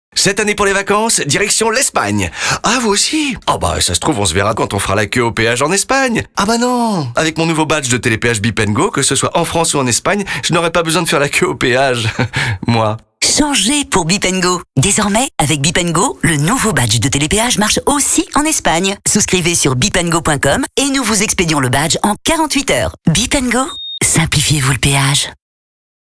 Bip&Go Espagne
Bip&Go - Studio : Mediameeeting Studios - Date de diffusion : juin et juillet - Campagne diffusée sur Locales de NRJ/Chérie Fm/RFM/Virgin Radio/Toulouse Fm/Wit FM/Sud Radio
Concepteur-Rédacteur : Eric Brégou.
Ingénieur du son prise de voix : Maxime Paillé.
Mixage : Eric Brégou.
Comédiens : Alexis Victor et Myriam Callas.